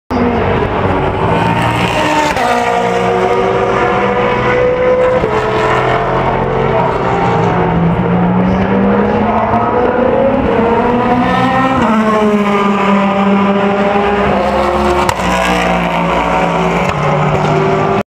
Stuart Hall Aston Martin V8 Vantage GTE Silverstone ELMS
Gulf Racing driver Stuart Hall in the Aston Mart Racing V8 Vantage GTE rounds Luffield corner during the European Le Mans Series race at Silverstone, April 2014.